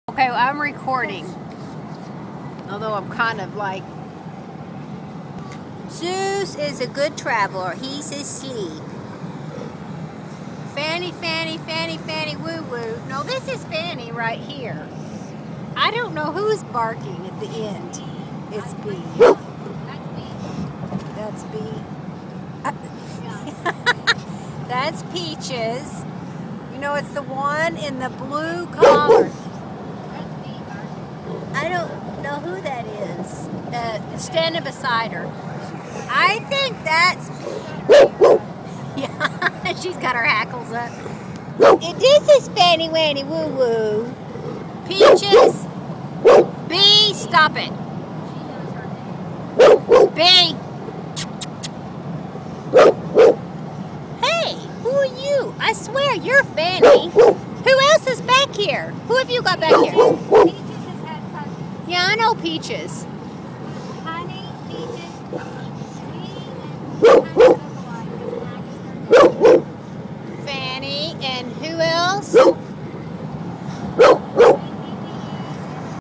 Fanny oh To Bee A Friend of God, Honey, Peaches
Ok. We figured out it was Peaches dpi g the barking. Zeus didn't care. Bee was trying to climb up front w me. All red Dogues w blue collars